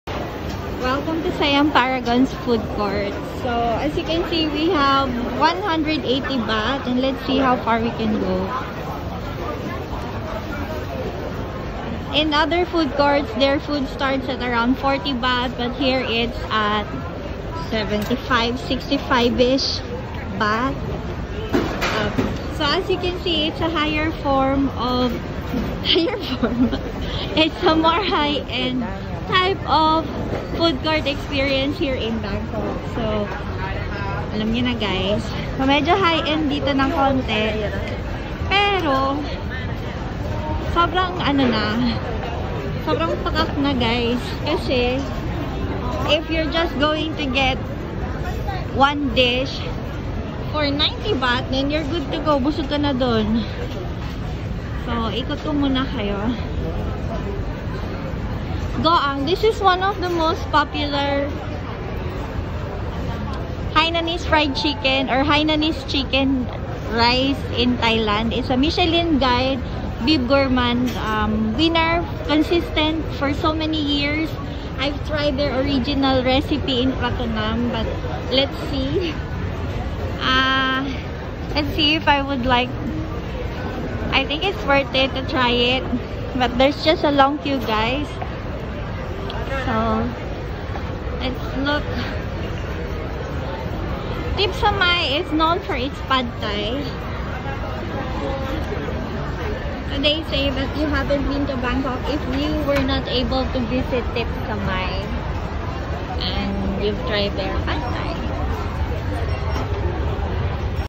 Bangkok Tour will not be complete without Food! Let's look at Siam Paragon's Food Court
Bangkok trip  will never be the same without ang visit to its food courts. You have to experience it!